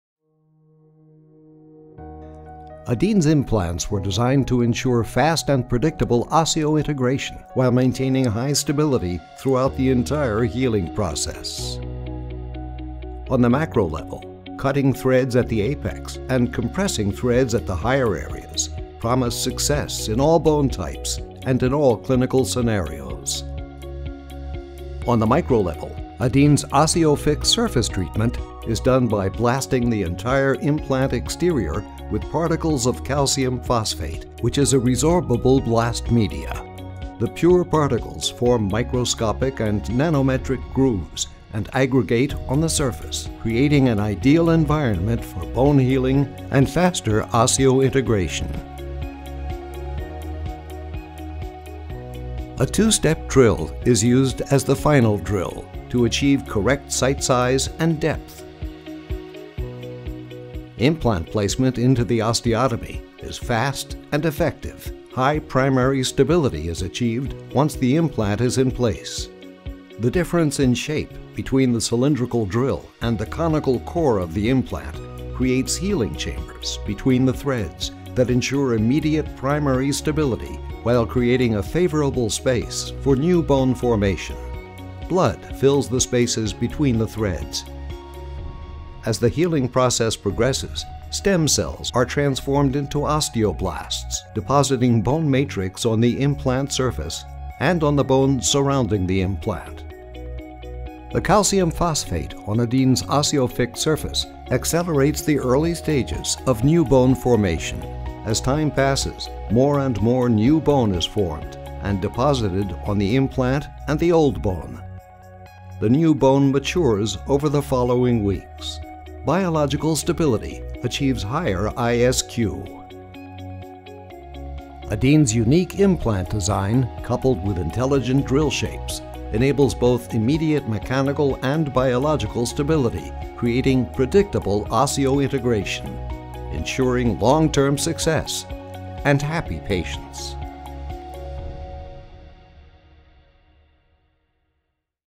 Adin Implants - Faster bone healing through science & design
Adin’s OsseoFix™ – Titanium implant surface treatment that uses gritting with calcium phosphate media that is debris free and has been designed to achieve the desired roughness levels needed for optimal osseointegration.
The OsseoFix™ is biocompatible.